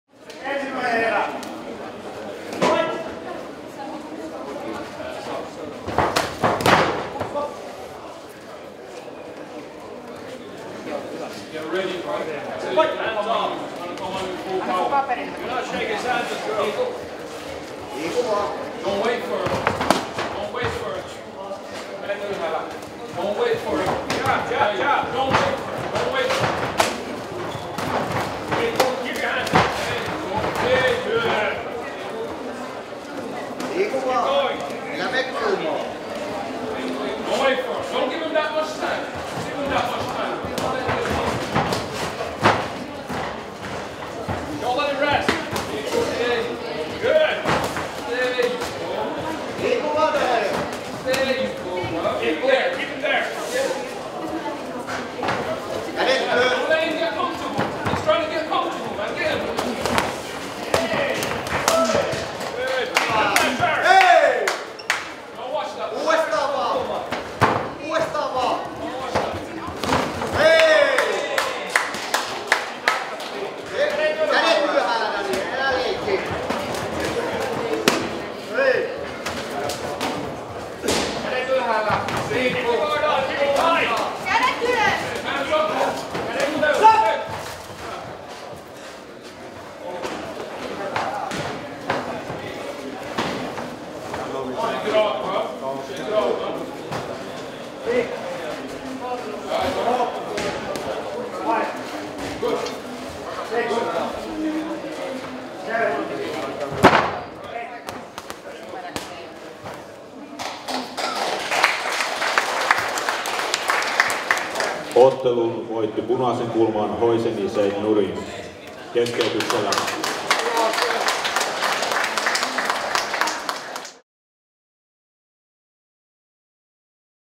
Lknv -71kg Hoseini Seyed Nuri JKB vs. Andrea Gasperi SSKF, RSC1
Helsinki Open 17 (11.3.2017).